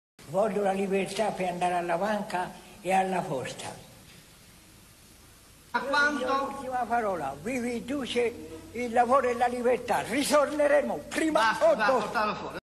MARIO VANNI: VIVA IL DUCE IL LAVORO E LA LIBERTÀ
Libertà di andare alla posta e alla banca.
Il compagno di merende del Pacciani da' spettacolo al processo sul Mostro di Firenze